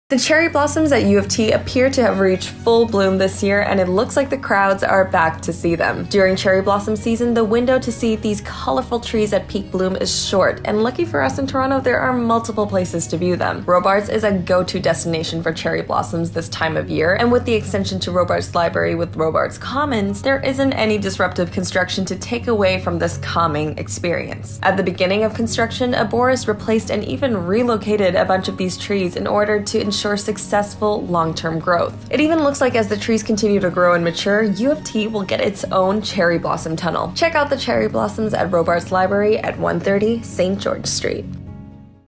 There are Cherry Blossoms at Robarts in Toronto
Cherry blossoms are in bloom at Robarts Commons in Toronto and the crowds are here to take photos